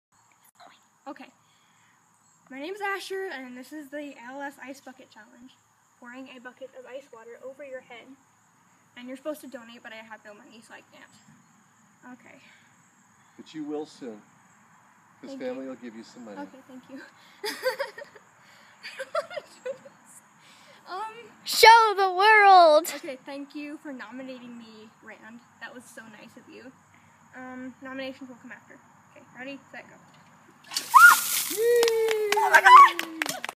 Asher does the als ice bucket challange